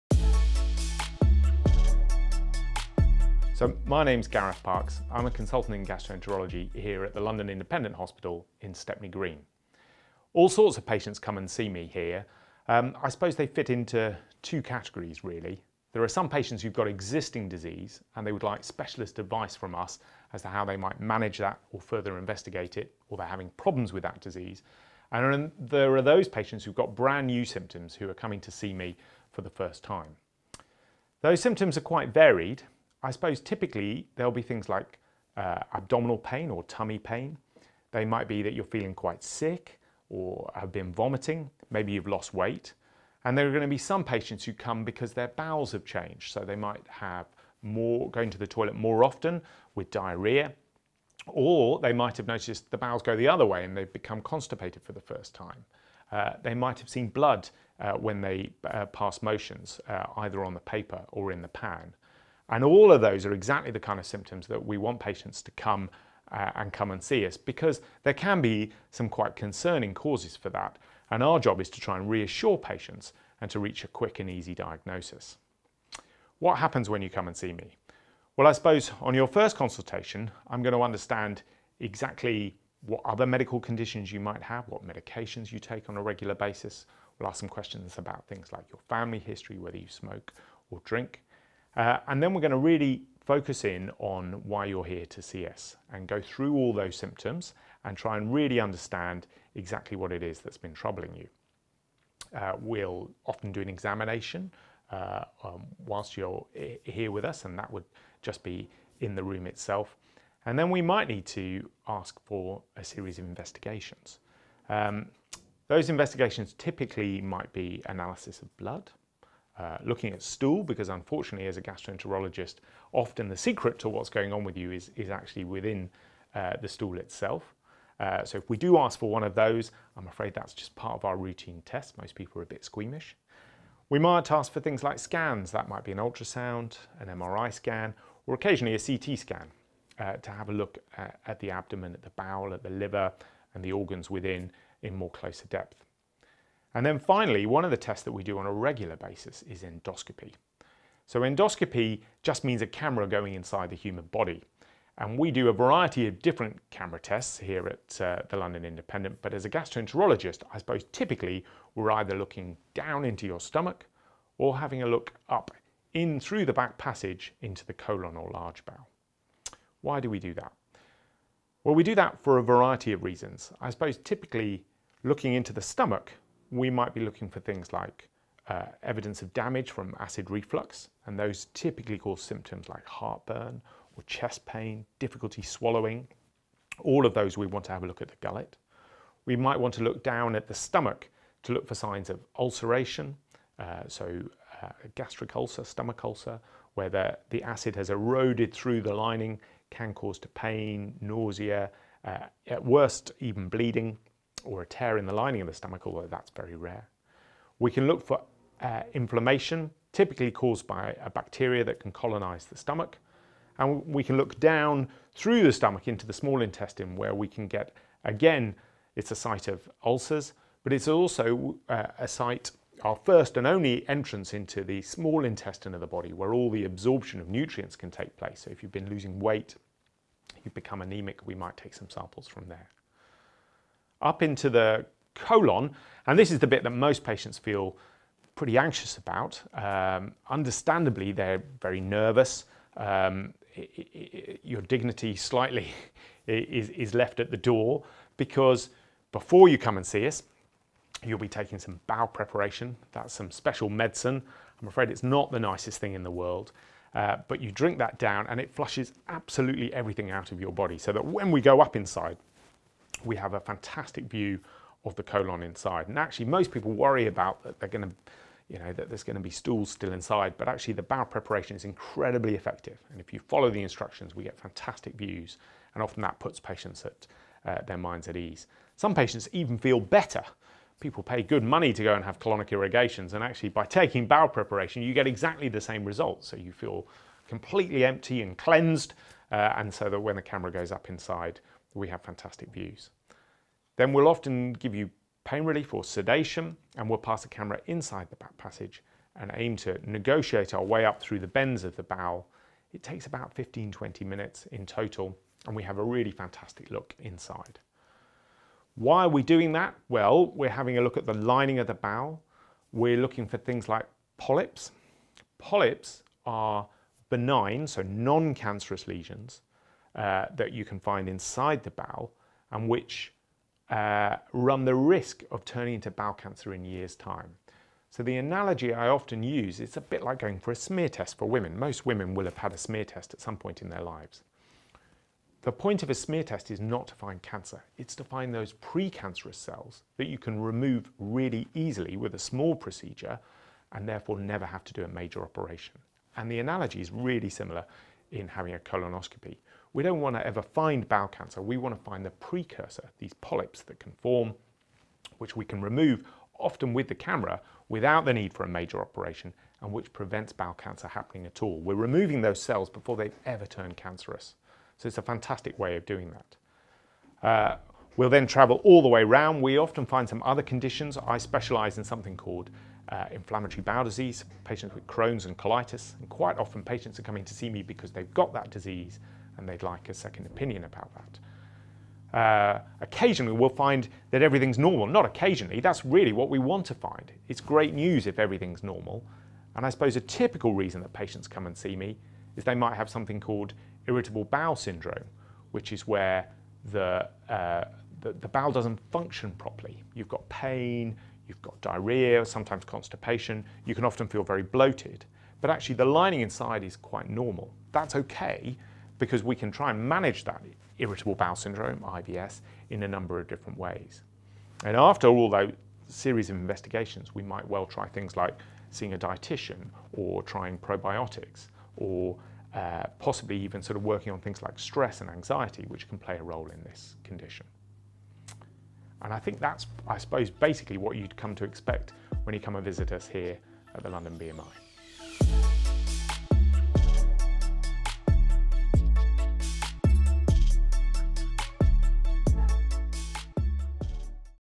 Bowel health and colonoscopies - Gareth Parkes - The London Independent
Dr Gareth Parkes, Consultant Gastroenterologist, explains the symptoms associated with bowel disorders and what to expect if a colonoscopy is needed.

For more information on Dr Parkes' practice and particular areas of interest, visit the